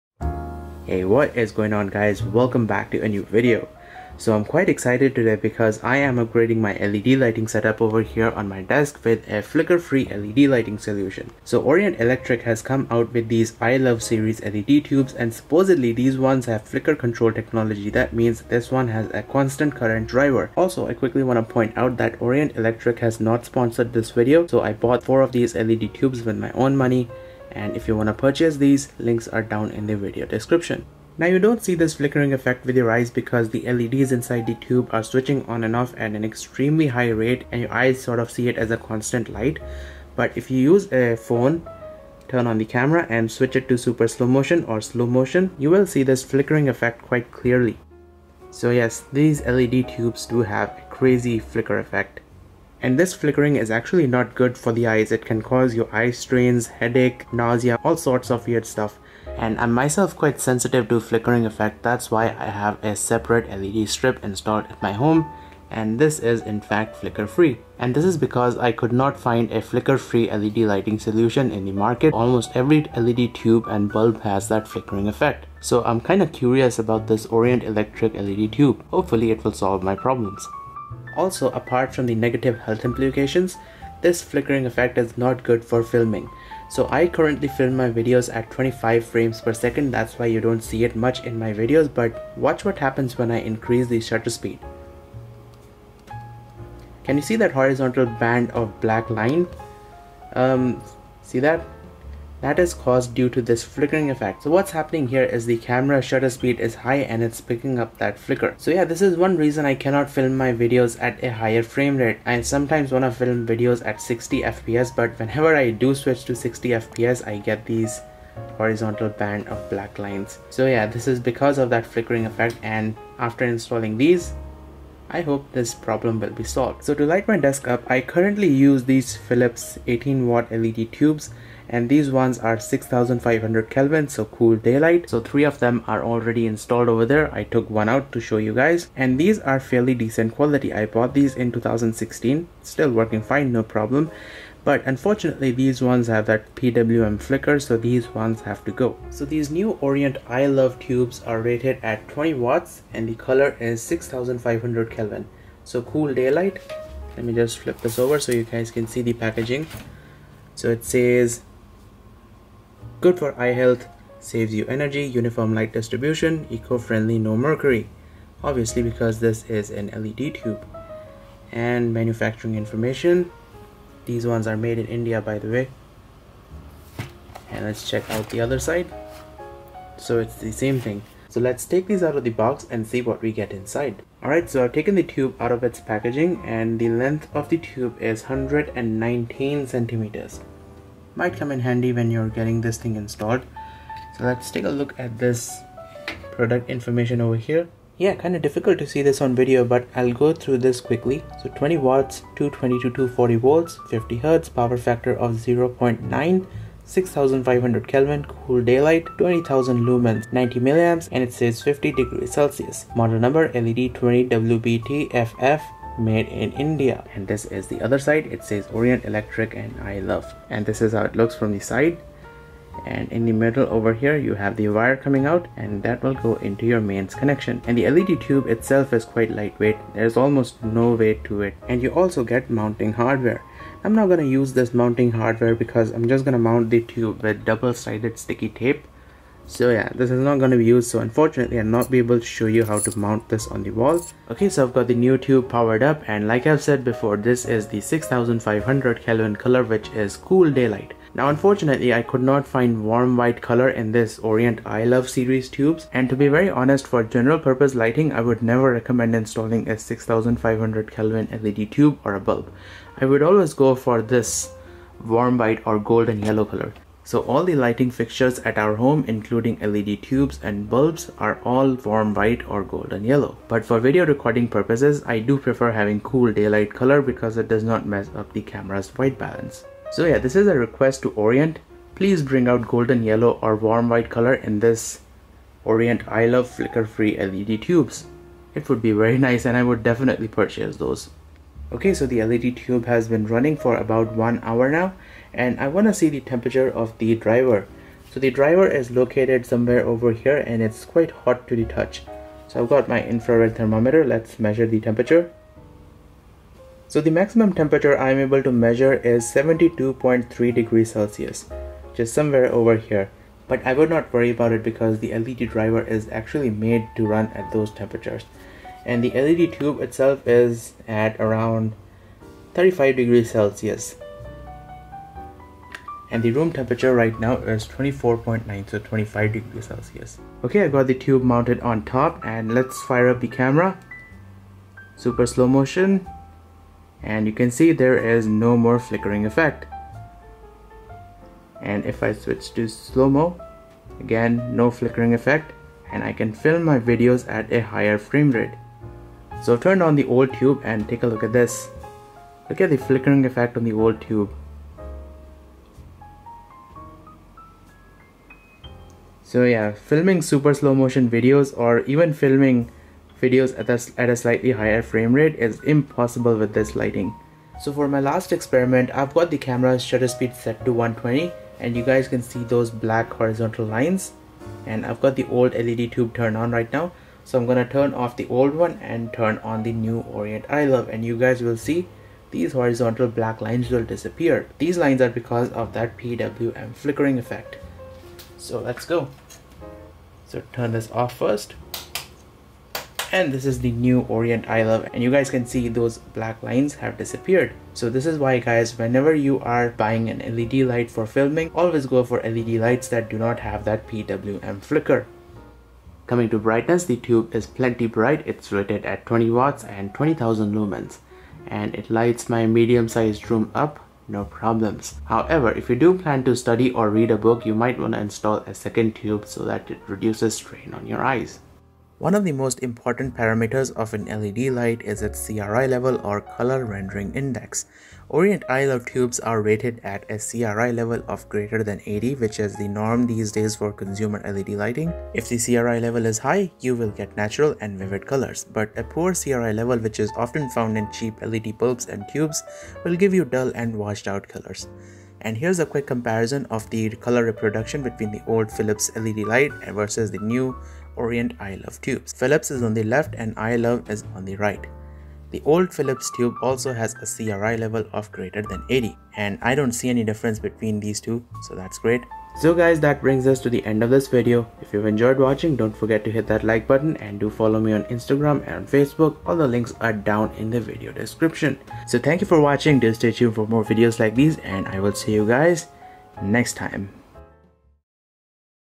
Orient EyeLuv flicker free LED tube
Today I have upgraded my desk lighting setup with new Flicker-Controlled 'EyeLuv' LED Tubes from Orient. You also get 'EyeLuv' flicker controlled LED bulbs.

Tube 20 watt